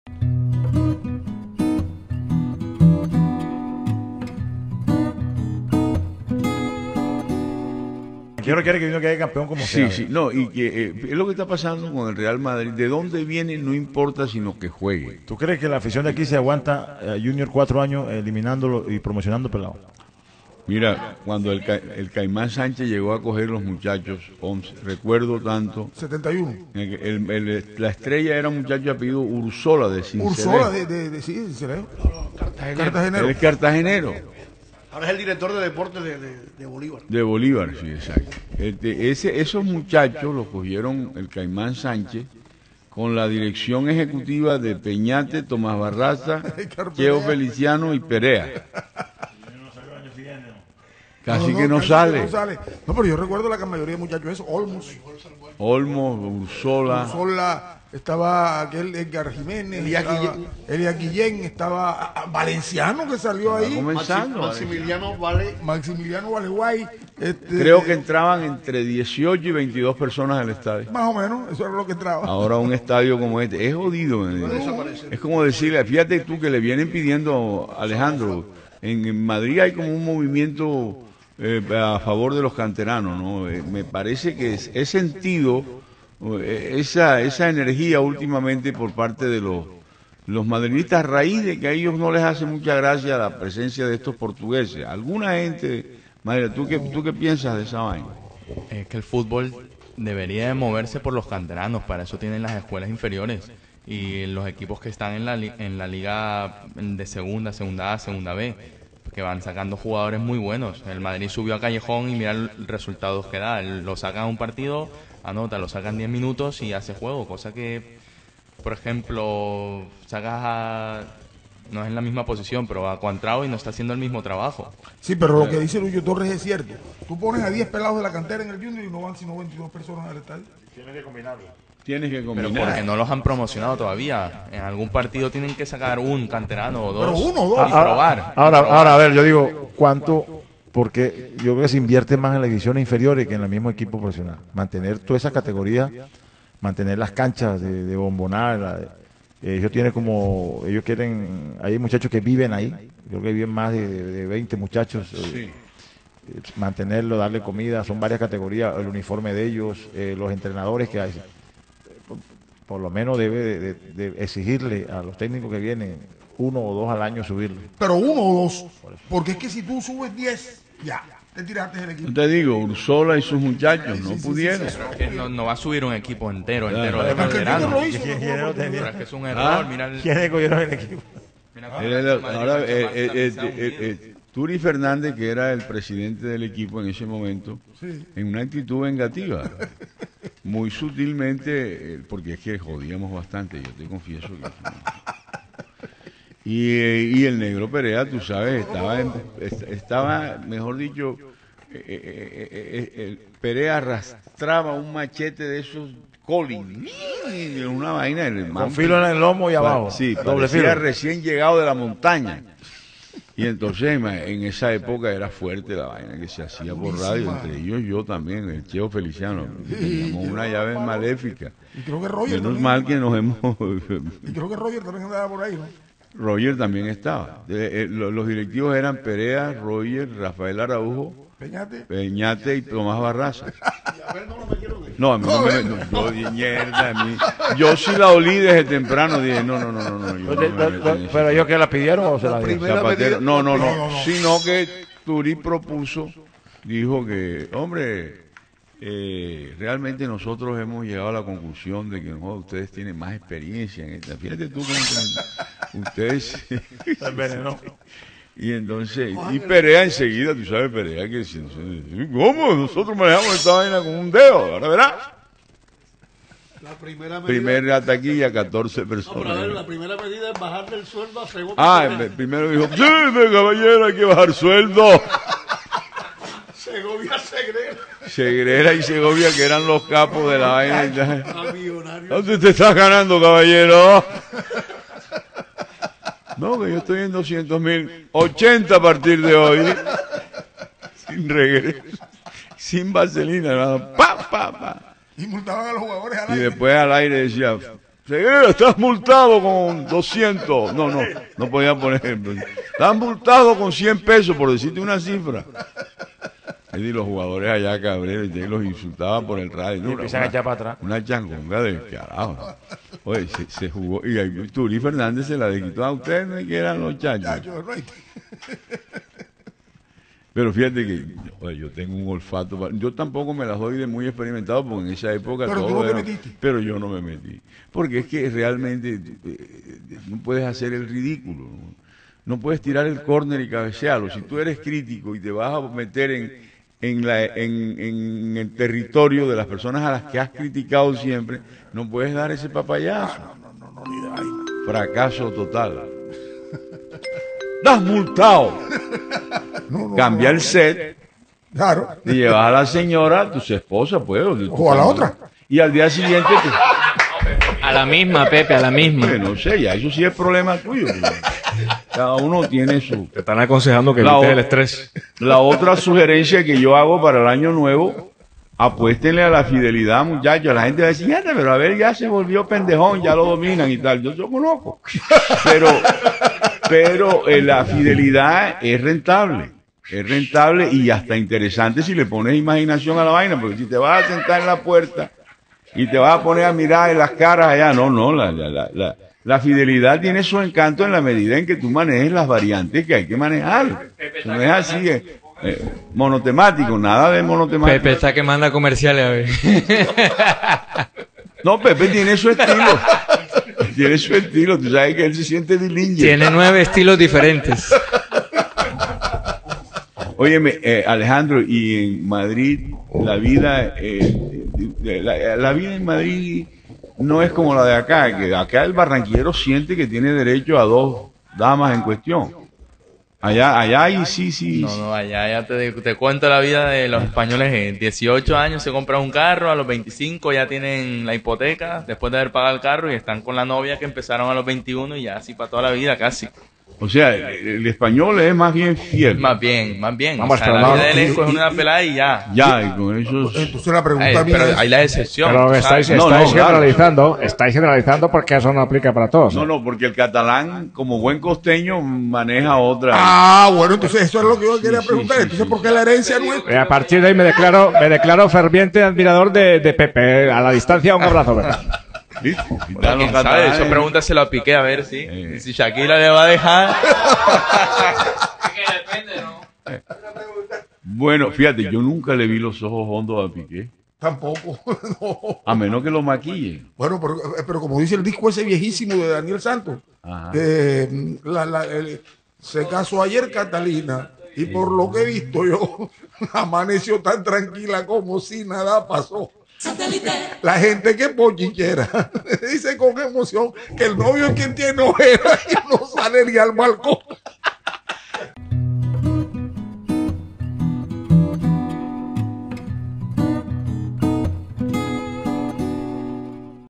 Remember Time: El Junior del 71, los gerentes de la radio, y la fidelidad.
Hoy en nuestro RememberTime nuestro eterno director y fundador Abel González Chávez comparte varias historias: "El Junior del 71, los gerentes de la radio, las divisiones menores, cambiar el set y la fidelidad.